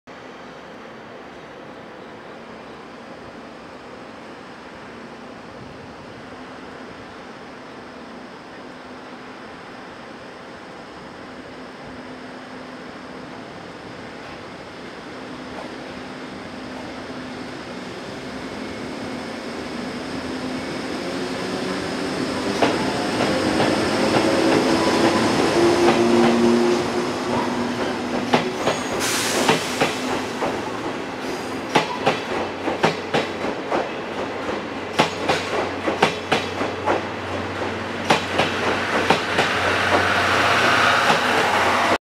【鉄道動画】184 山陽線 EF65-1133 牽引 臨時快速 やまぐちDCオープニング1号 新山口行き 9531レ 広島駅 2番 ホーム 発車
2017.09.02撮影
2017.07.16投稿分